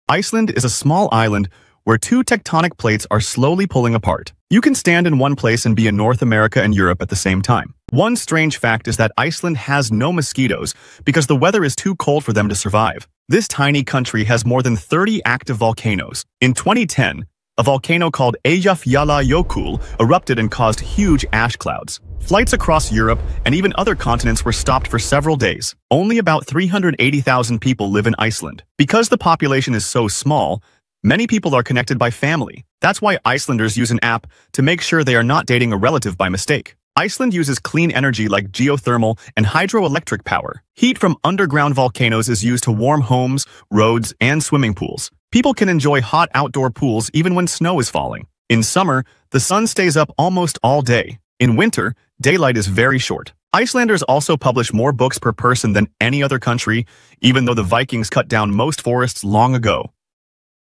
Why Iceland Is the Strangest Country on Earth 🇮🇸
Iceland sits between the North American and European tectonic plates — and they’re slowly pulling apart. You can literally stand on two continents at the same time.

This country has no mosquitoes, over 30 active volcanoes, and uses underground heat to warm homes, roads, and swimming pools. Even with snow falling, people swim outside.

With only 380,000 people, Icelanders even use an app to avoid dating relatives. Add midnight sun, long winter darkness, and record-breaking book publishing — and Iceland becomes one of the strangest places on Earth.